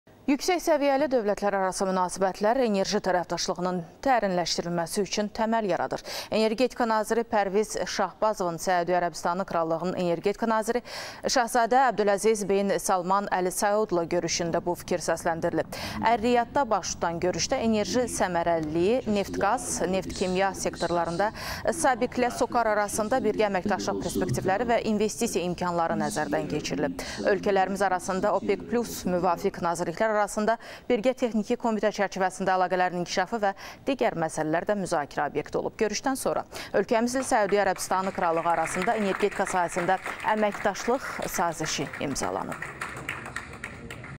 Azərbaycanla Səudiyyə arasında saziş: energetika sahəsində sıx əməkdaşlıq qurulur
SƏUDİYYƏ İLƏ YENİ ƏMƏKDAŞLIQ SAZİŞİ İMZALANDI